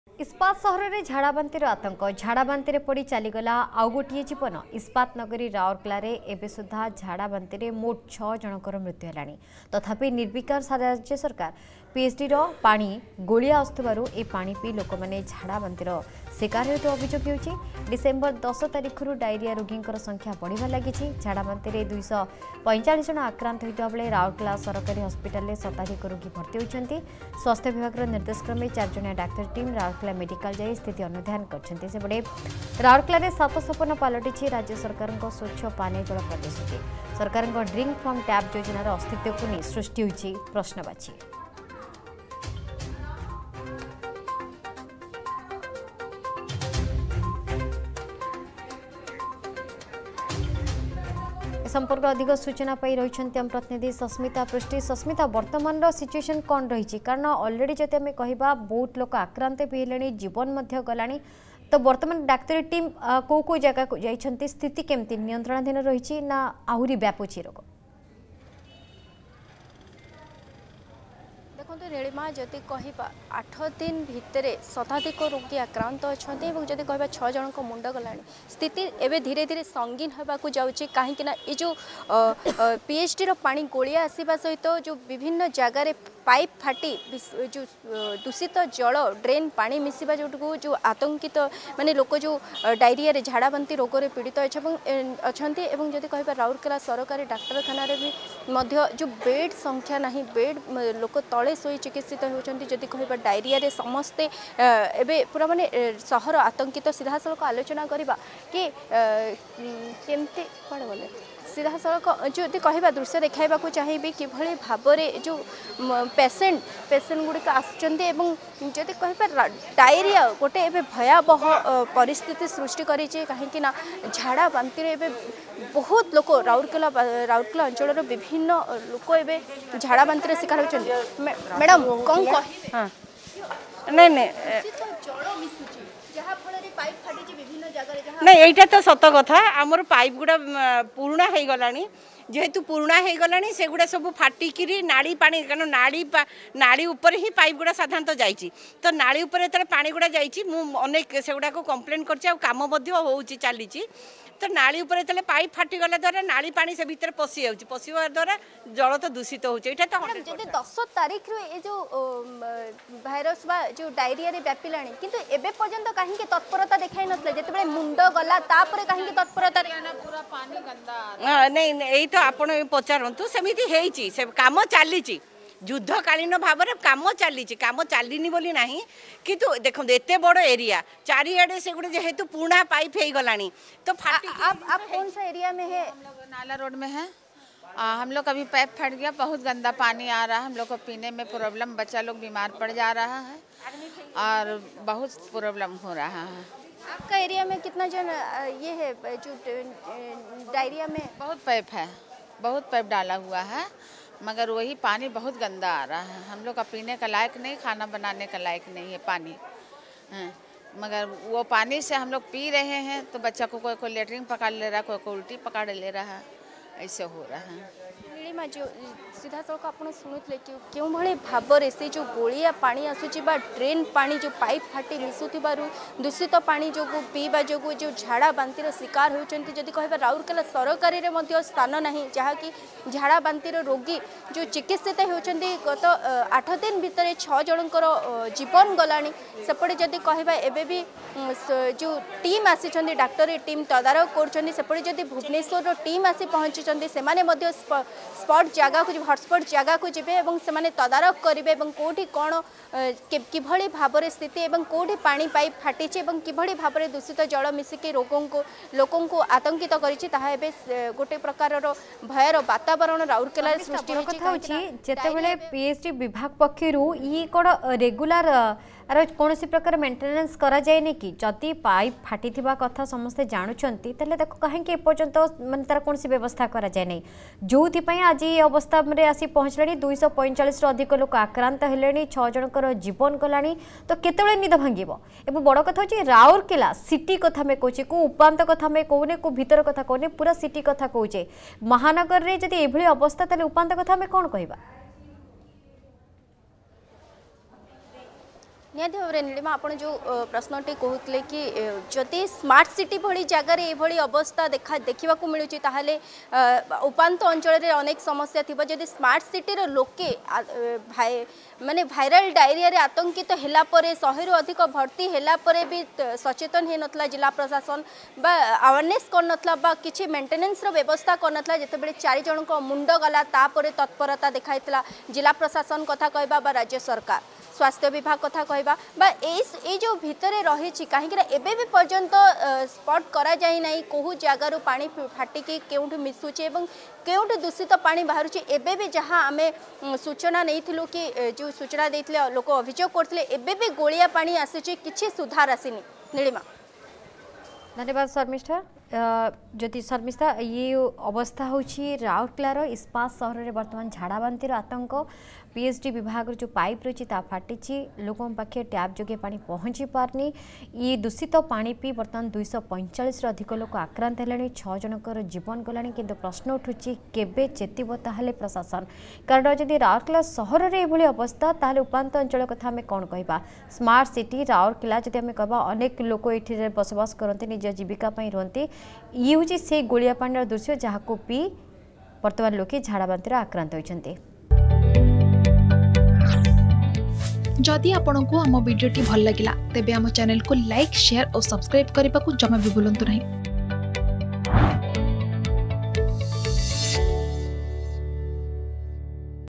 ଇସ୍ପାତ ସହରରେ ଝାଡ଼ାବାନ୍ତି ଆତଙ୍କ || Rourkela || Diarrhea
ଇସ୍ପାତ ସହରରେ ଝାଡ଼ାବାନ୍ତି ଆତଙ୍କ । ଦୂଷିତ ପାଣି ପିଇ ଗଲା ଆଉ ଗୋଟେ ଜୀବନ । ଏବେ ସୁଦ୍ଧା ୬ ଜଣଙ୍କ ମୃତ୍ୟୁ । ସାତ ସପନ ପାଲଟିଛି ସ୍ବଚ୍ଛ ପାନୀୟ ଜଳ ପ୍ରତିଶ୍ରୁତି ।